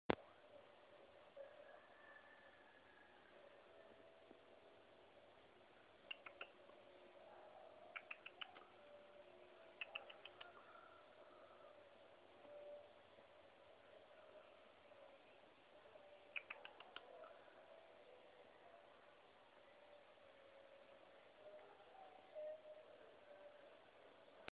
Adestramento de cães de rua.
Meu pai e suas técnicas de adestrador.
Tombe a cabeça para o lado porque eu não sei virar vídeos.